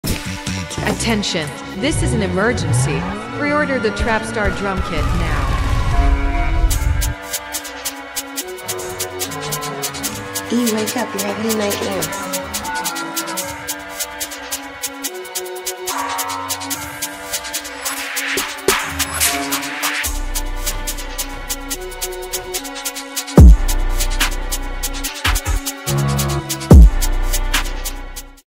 [350+] NY DRILL DRUM KIT 2023 "808STAR" Kay Flock x Lil Mabu x Sha Ek
CONTENT

-70 EXTRAS
(Oneshots, Mixers etc.)
-39 808s
-22 CLAPS
-34 FX
-41 COUNTERSNARES
-24 FILLS
-10 KICKS
-16 PERCS
-30 SNARES
-42 VOX

This drill kit contains everything you need to start or if you are experienced with Drill.If you're a beginner or a intermediate Producer, then this drill kit is perfect for you! With this kit, you'll be able to make the coldest beats. grab your [800+] NY DRILL KIT 2023 "808 WARS" Kay Flock x B Lovee x Sha Ek (EVERYTHING YOU NEED!)